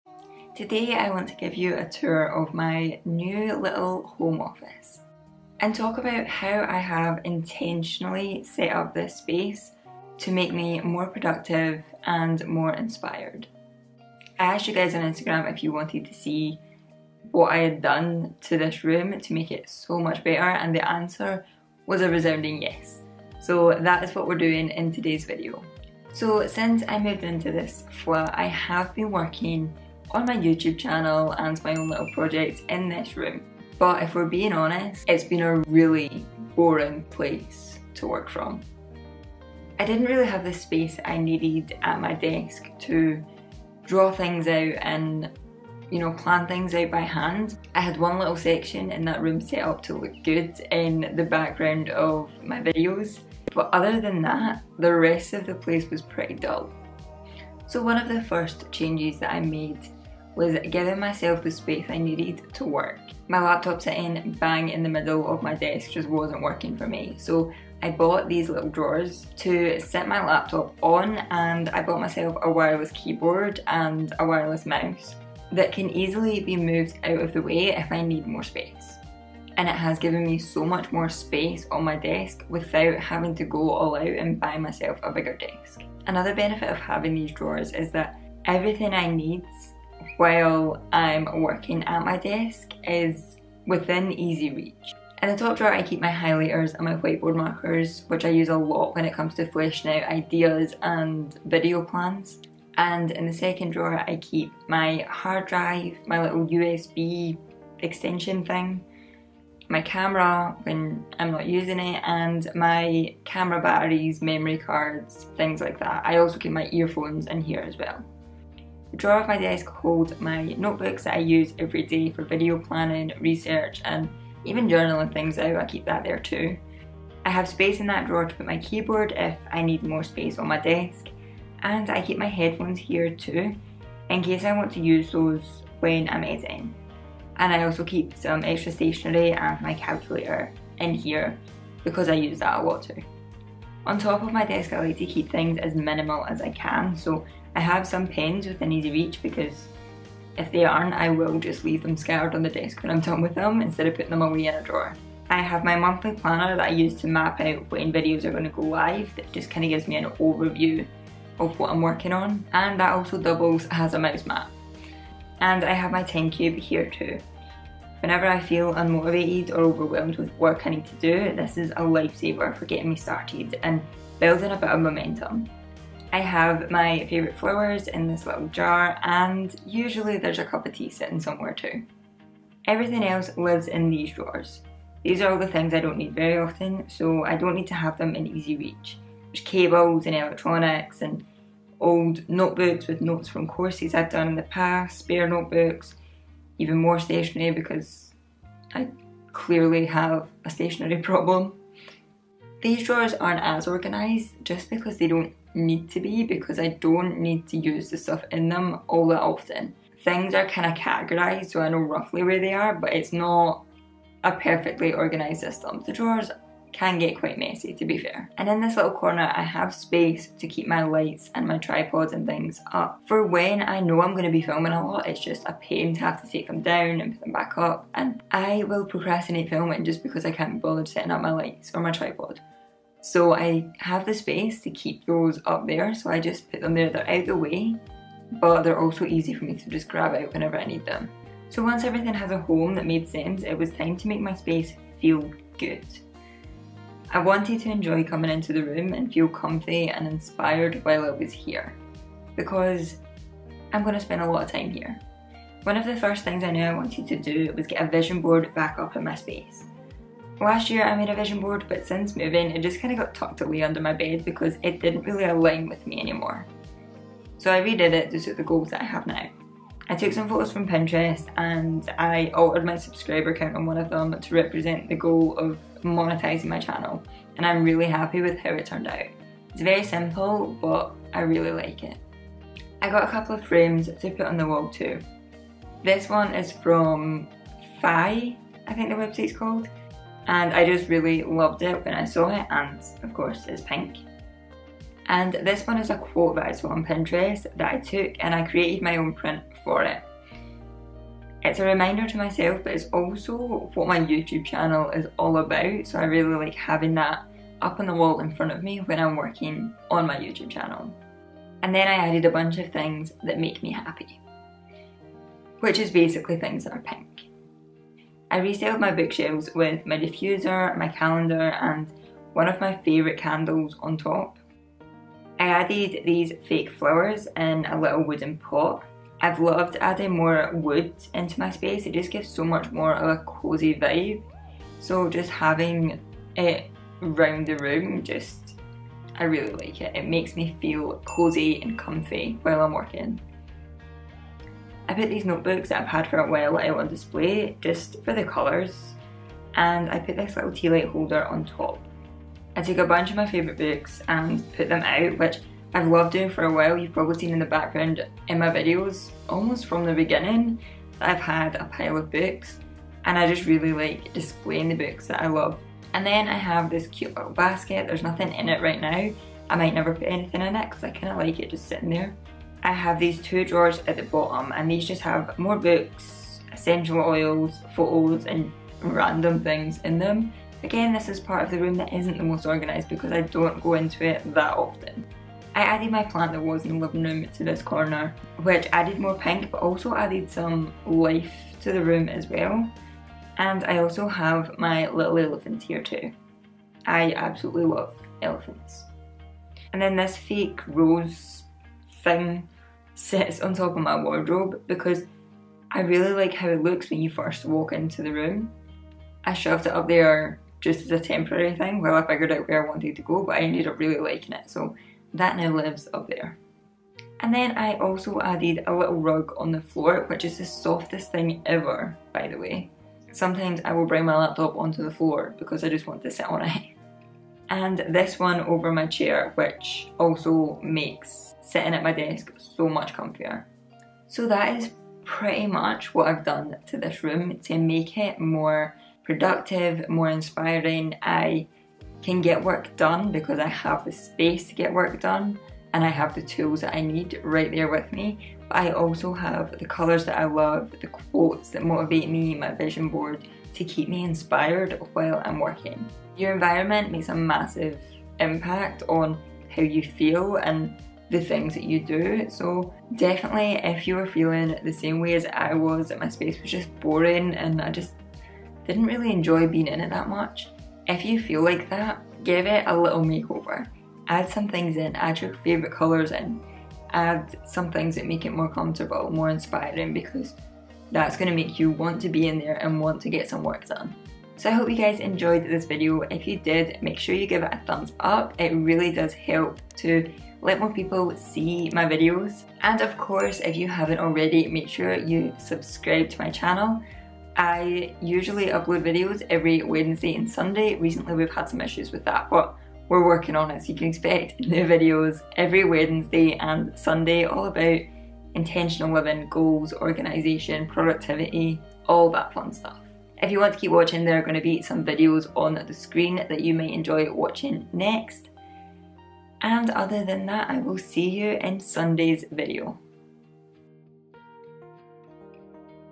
how to make your desk productive + inspiring
this video is all about how to make your desk productive and make your desk inspiring.

Recently my desk/home office was feeling pretty stagnant and I was struggling to get things done and be productive in that space. I decided to film the process of transforming my home office and creating a productive space to work on my goals in. I love watching videos about a youtuber home office or creating a productive study space (the stationary geek inside me gets hella excited) so I wanted to make a video of my own home office transformation for you. Whether you want to know how to create a productive space, how to make your desk more inspiring, how to make your desk more organised or even just want to see my new lil office, this video is for you!

☁ If you liked this video, here’s a few similar videos you might like too

☁ Things I mentioned in the video + helpful resources

bamboo desk drawers
time cube
- on my bookshelf -
daring greatly by brene brown
good morning, good life by amy landino
resonate by alex wolf
rising strong by brene brown
the 5 second rule by mel robbins
atomic habits by james clear

☁ If you want to hang out on another platform

☁ Music

☁ About

This channel is all about making it easier to live an intentional, designed with purpose kinda lifestyle.